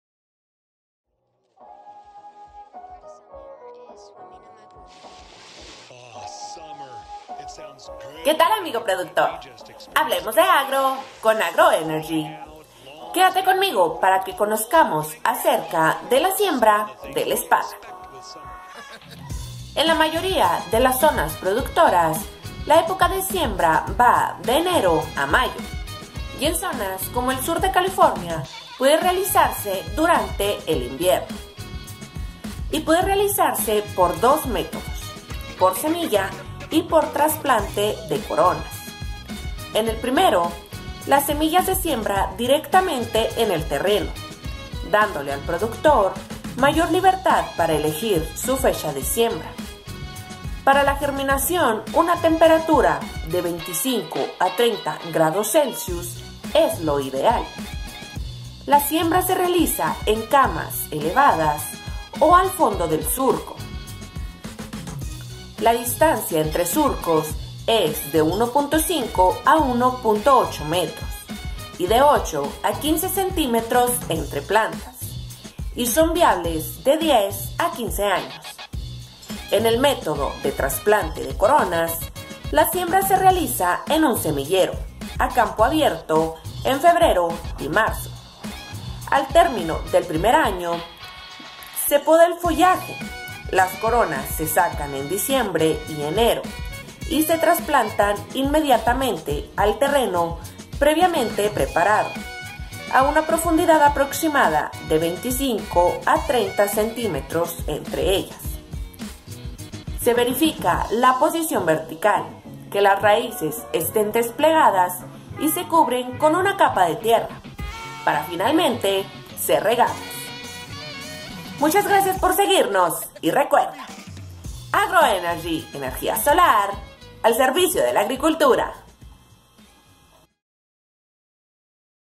La siembra del esparrago #044
¿Cual es la mejor fecha para sembrar esparragos?
Acompáñanos para saber mas Visita nuestra página oficial de Facebook

Tecnologías sustentables al servicio de la Agricultura